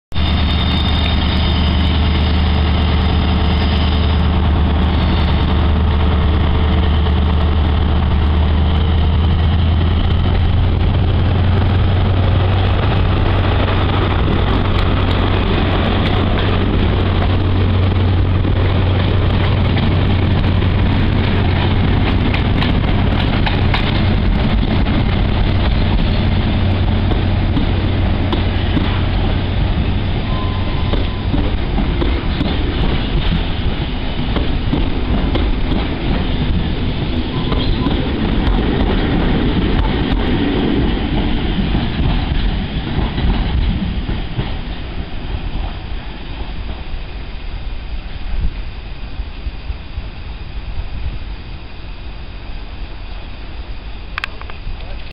network rails 43014 and class 31 31602 rumble through newcastle 15/06/11
network rails  43014 and class 31 31602 rumble through newcastle plus a departure from GC hst 43468 and 43465 15/06/11